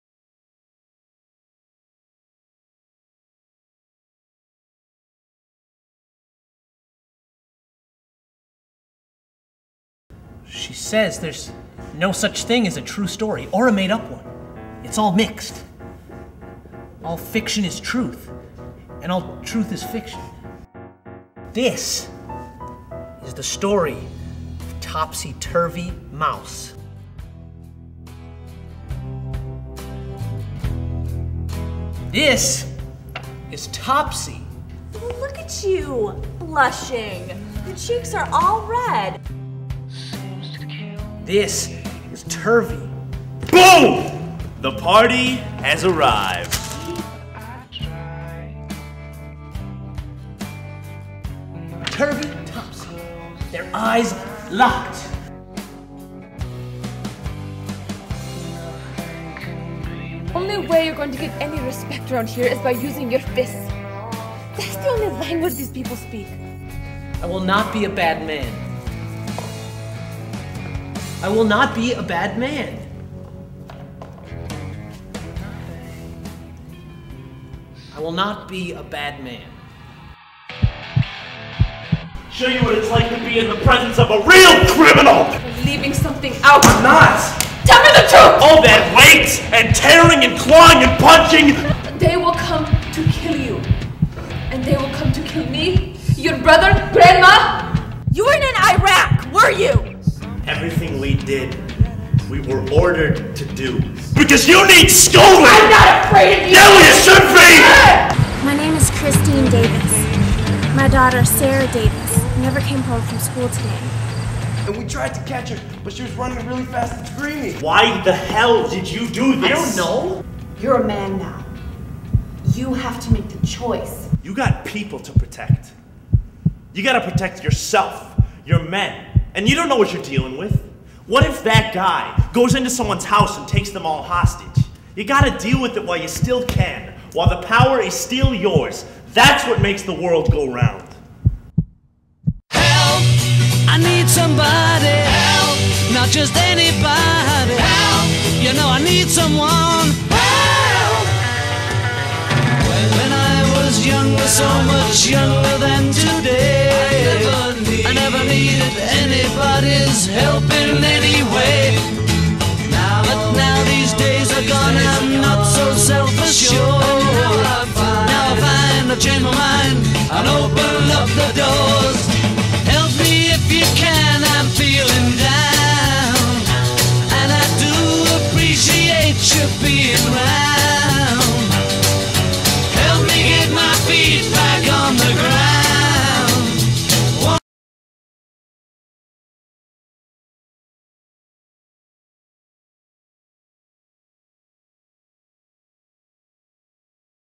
Gustavus Adolphus College presents Topsy Turvy Mouse
A teaser trailer for Gustavus Adolphus College's production of Topsy Turvy Mouse.

Directed by Amy Seham

Written by Peter Gil-Sheridan

-Watch in high quality, it looks a little better.